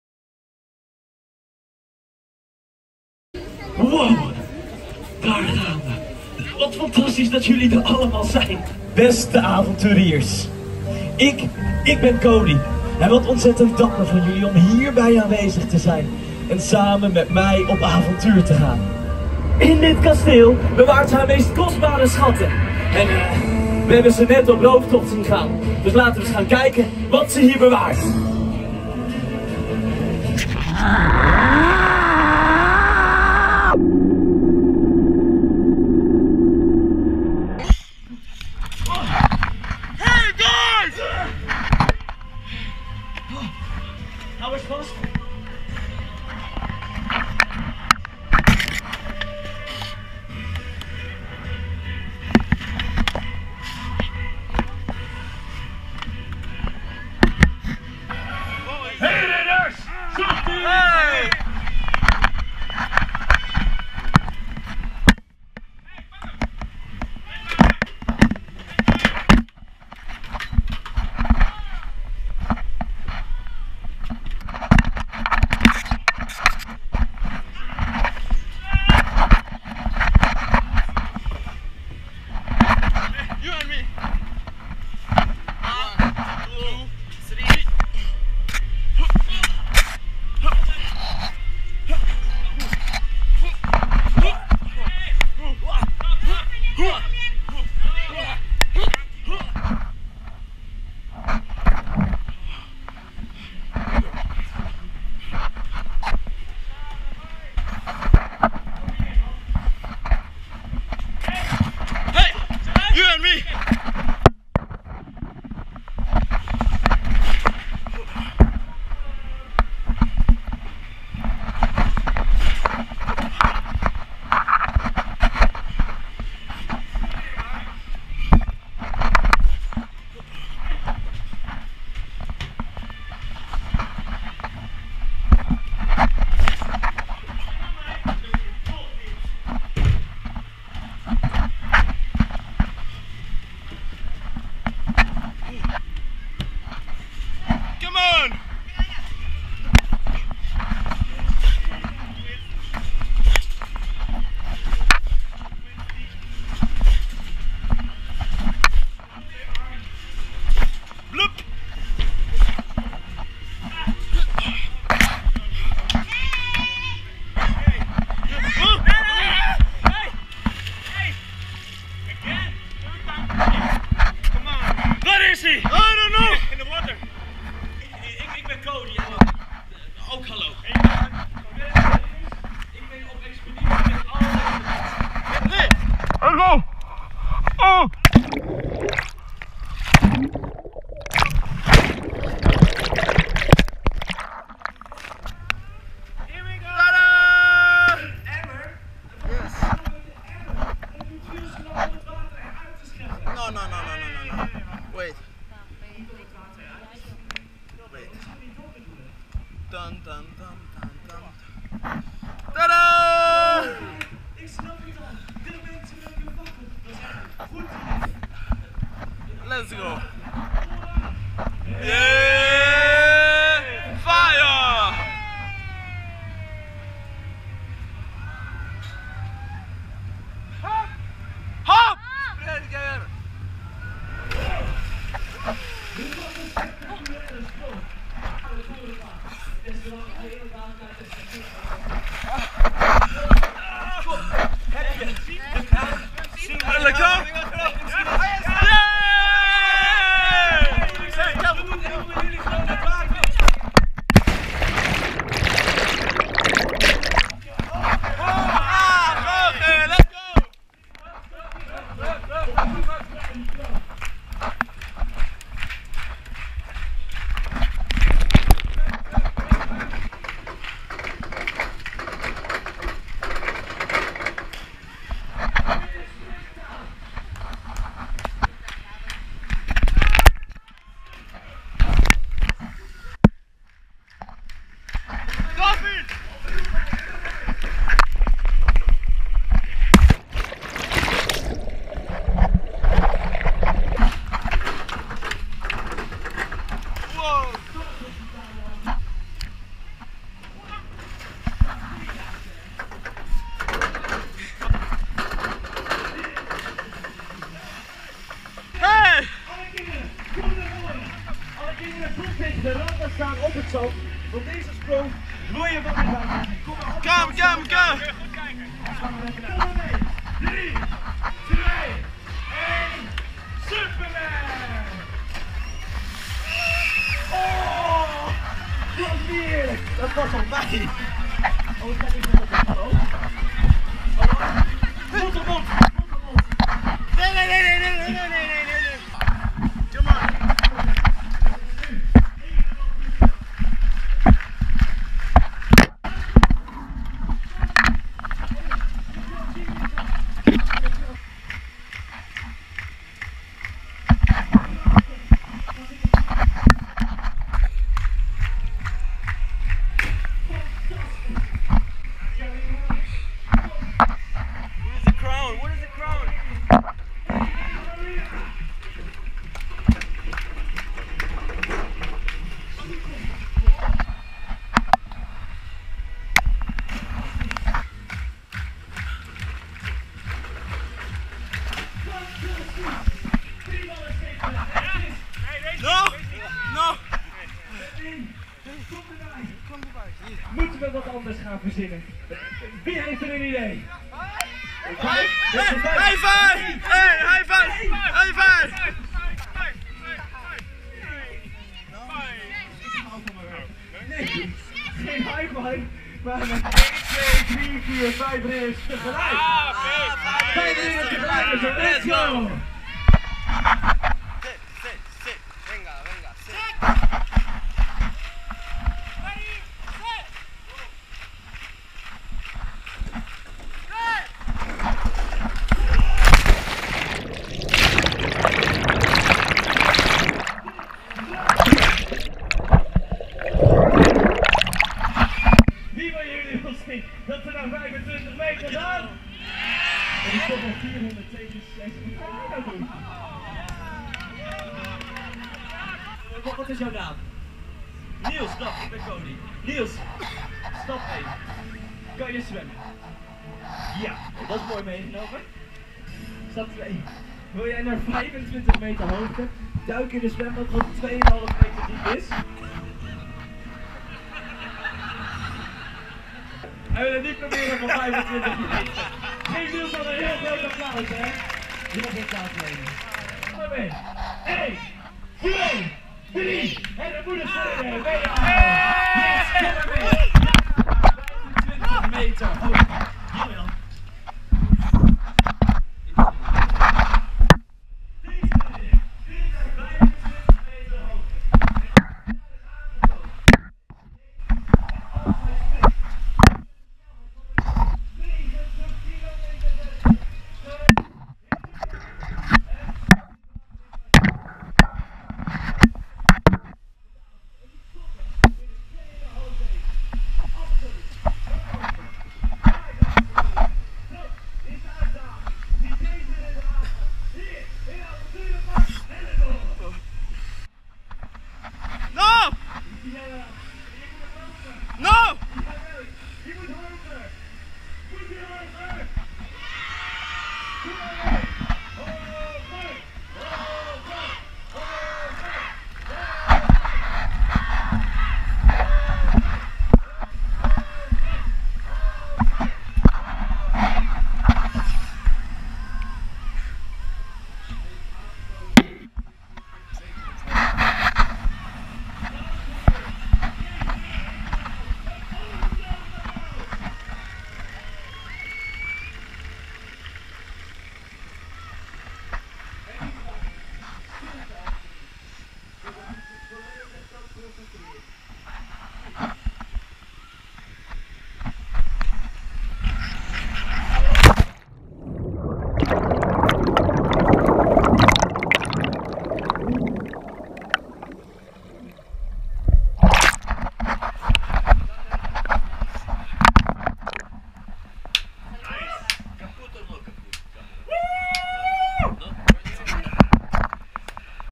Stuntshow Hellendoorn (2019) - ON STAGE VIEW
De High Dive Stuntshow 'De Drakenridders' vanaf het perspectief van de stuntmensen. Uniek perspectief van de stuntshow uit Hellendoorn (2019)